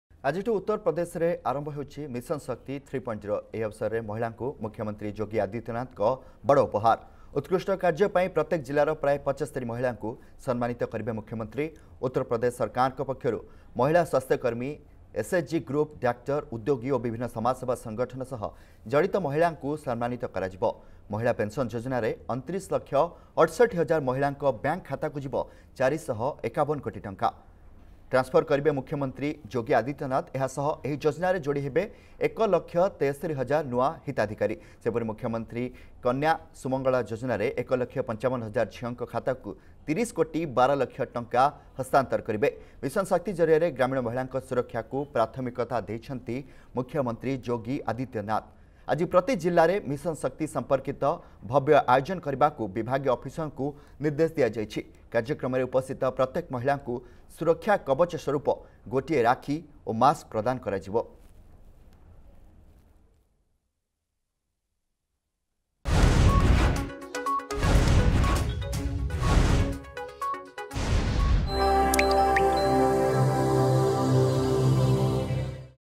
ଆଜିଠୁ ଉତ୍ତରପ୍ରଦେଶରେ ଆରମ୍ଭ ହେଉଛି ମିଶନ ଶକ୍ତି 3.0 | Argus News
ଆଜିଠୁ ଉତ୍ତର ପ୍ରଦେଶରେ ଆରମ୍ଭ ହେଉଛି ମିଶନ ଶକ୍ତି 3.0 । ଏହି ଅବସରରେ ମହିଳାଙ୍କୁ ମୁଖ୍ୟମନ୍ତ୍ରୀ ଯୋଗୀ ଆଦିତ୍ୟନାଥଙ୍କ ବଡ଼ ଉପହାର ।